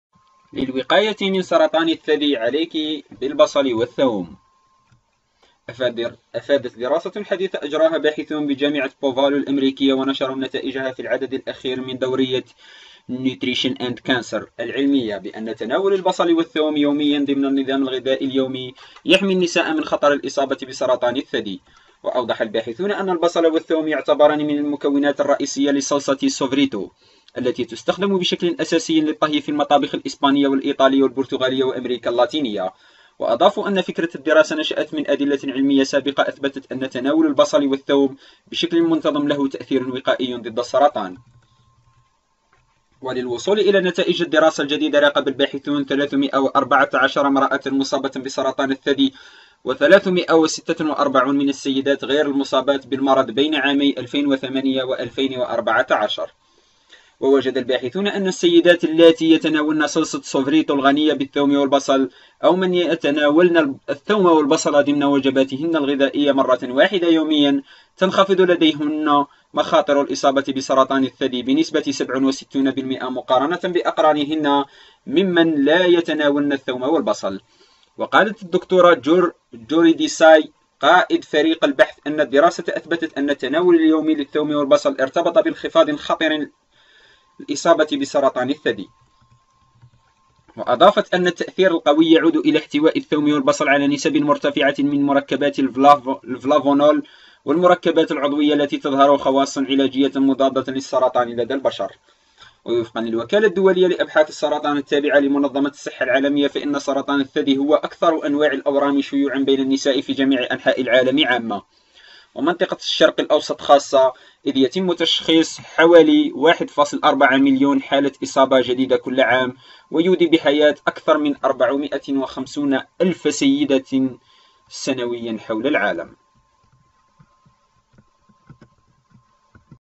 للوقاية من سرطان الثدي ... عليكِ بالبصل والثوم
أفادت دراسة حديثة أجراها باحثون بجامعة بوفالو الأميركية ونشروا نتائجها، في العدد الأخير من دورية (Nutrition and Cancer) العلمية، بأن تناول البصل والثوم يوميا، ضمن النظام الغذائي اليومي، يحمي النساء من خطر الإصابة بسرطان الثدي.

وأوضح الباحثون أن البصل والثوم يعتبران من المكونات الرئيسية لصلصة "سوفريتو" التي تستخدم بشكل أساسي للطهي في المطابخ الإسبانية والإيطالية والبرتغالية وأميركا اللاتينية.